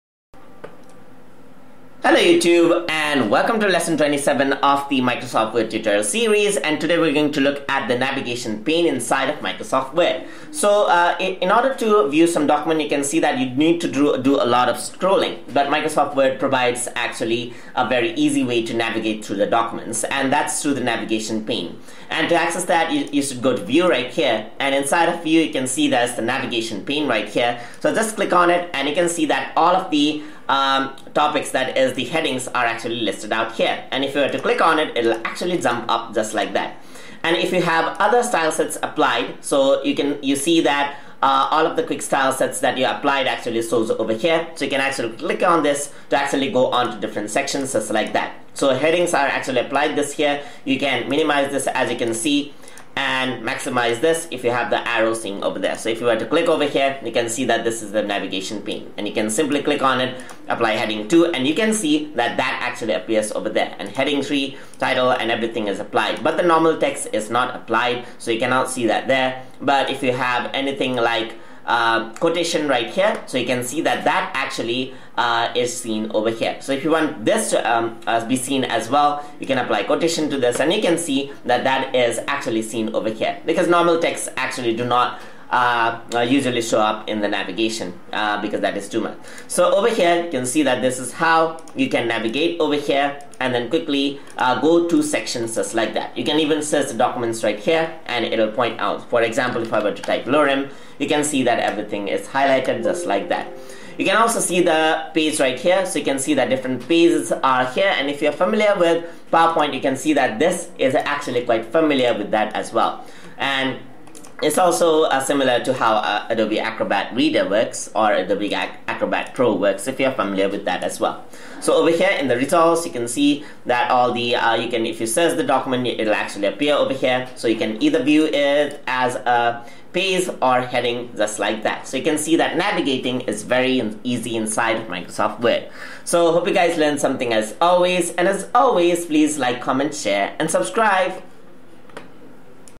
MS Word Tutorial - Lesson 27 - Navigation Pane and Heading Styles
In this tutorial we will be discussing on Navigation Pane and Heading Styles inside of MS Word.
Learn the basics of using Word, including how to format a document, format text and make use of editing tools.

This tutorial was made using Word 365, but is applicable to older and newer versions of Word as well